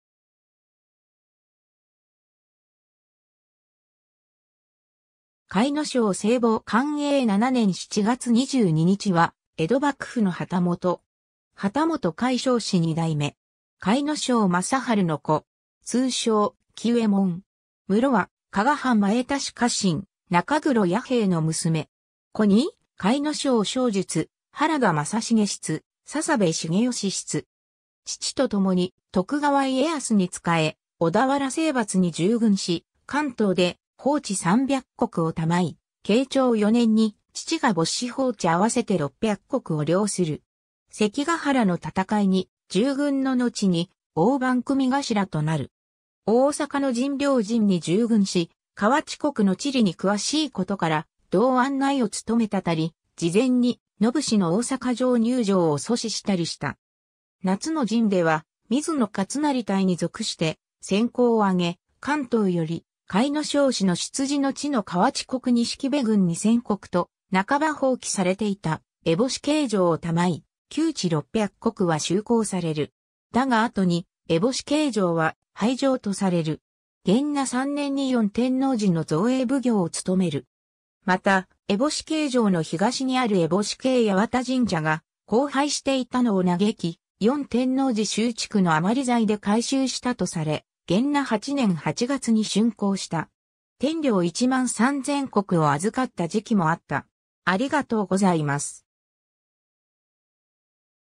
甲斐庄正房
甲斐庄 正房 （かいのしょう まさふさ、永禄7年（1564年） - 寛永7年7月22日（1630年8月20日））は江戸幕府の旗本。
旗本甲斐庄氏2代目。
甲斐庄正治の子。
通称、喜右衛門（7代正壽まで同名を名乗る）。
室は加賀藩前田氏家臣中黒弥兵衛の娘。
子に甲斐庄正述、原田正成室、雀部重良室。
 父とともに徳川家康に仕え、小田原征伐に従軍し、関東で封地300石を給い、慶長4年（1599年）に父が没し封地合わせて600石を領する。
関ヶ原の戦いに従軍ののちに大番組頭となる。
大坂の陣両陣に従軍し、河内国の地理に詳しいことから道案内をつとめたたり、事前に野武士の大坂城入城を阻止したりした。
夏の陣では、水野勝成隊に属して戦功をあげ、関東より甲斐庄氏の出自の地の河内国錦部郡2000石と半ば放棄されていた烏帽子形城を賜い、旧知600石は収公される。
だが後に烏帽子形城は廃城とされる。
元和3年（1617年）に四天王寺の造営奉行を務める。
また、烏帽子形城の東にある烏帽子形八幡神社が荒廃していたのを嘆き、四天王寺修築の余材で改修したとされ、元和8年（1622年）8月に竣工した。
 天領13000石を預かった時期もあった。